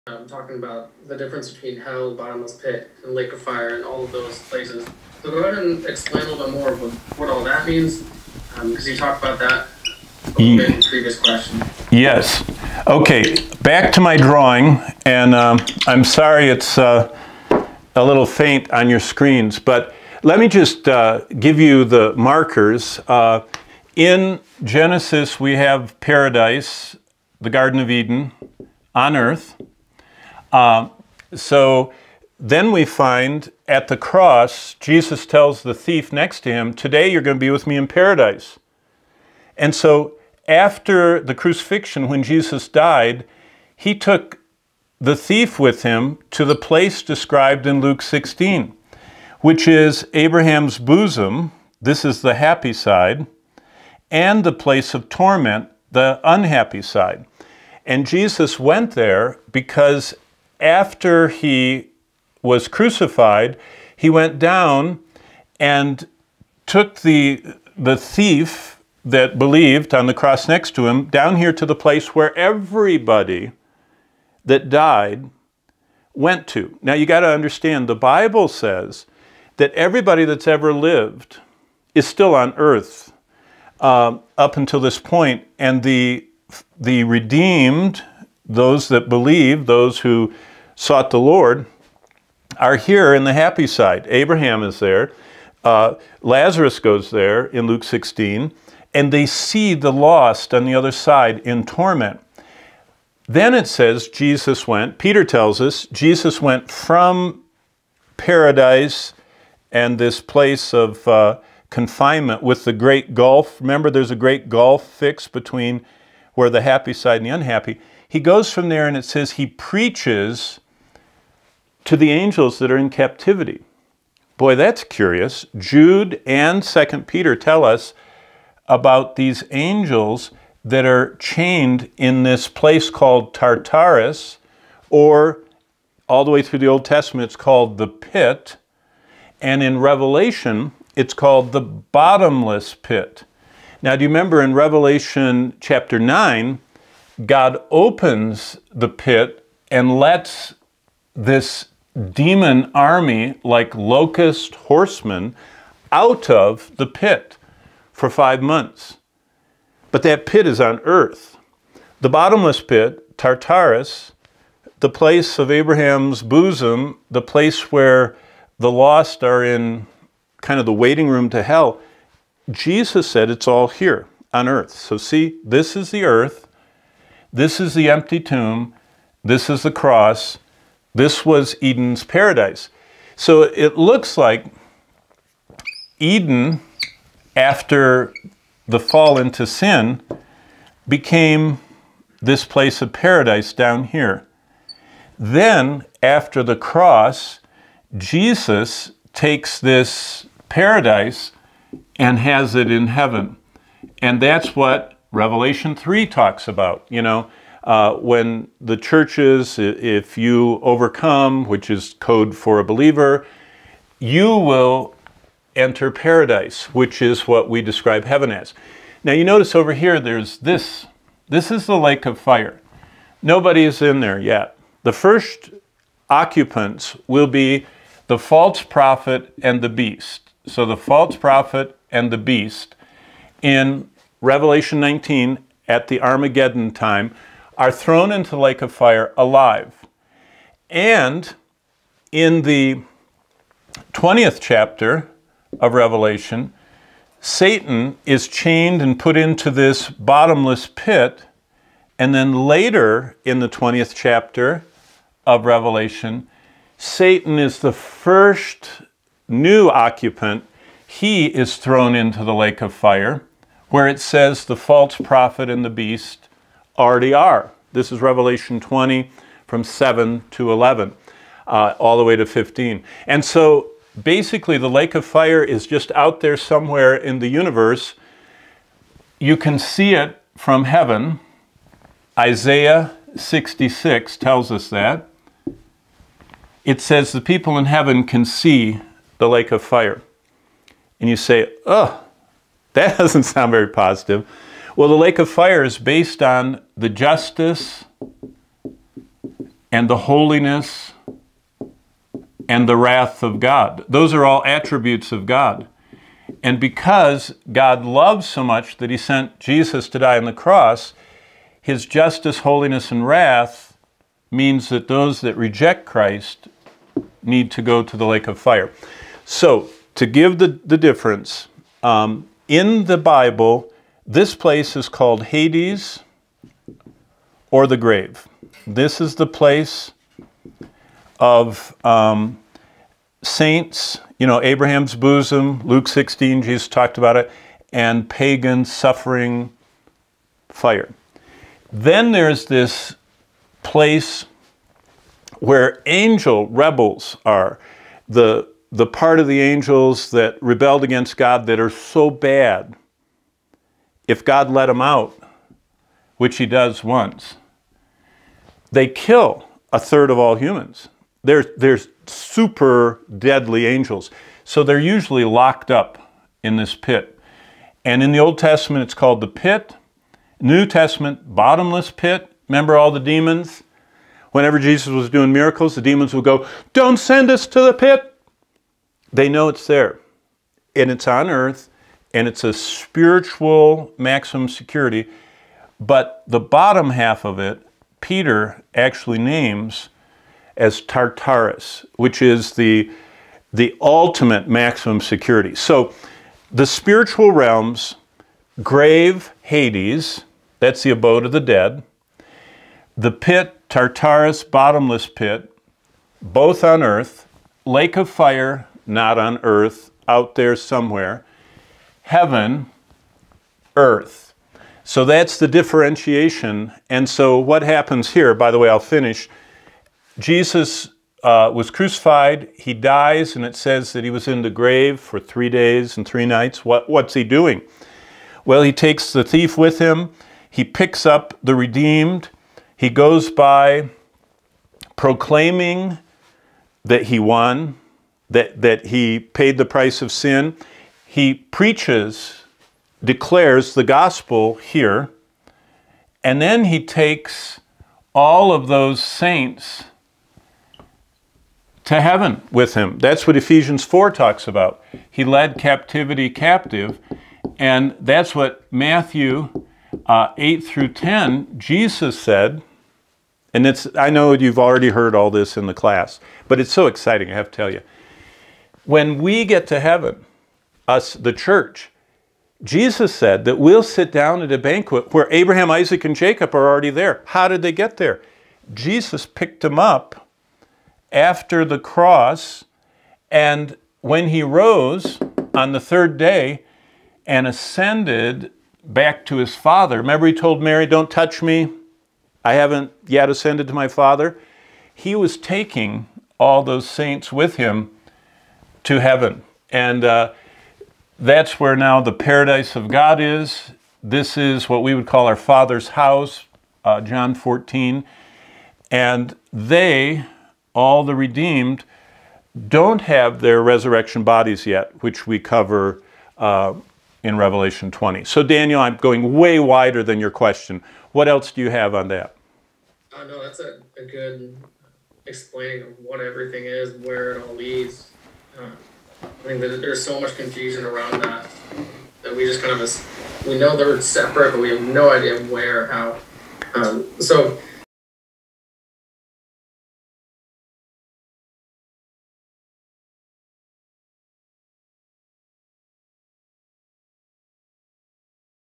Q&A: HADES, PARADISE, BOTTOMLESS PIT & LAKE OF FIRE
Now that the “Exploring Revelation” class is completed, and posted online—the questions are rolling in by the scores, actually by the hundreds. In the next few sessions, we are taking “Live” questions from students to us in our virtual classroom. Each Q&A session features an answer to a question formed from dozens of variations that all refer to a similar topic. Thanks SO much for your thoughtful, insightful questions.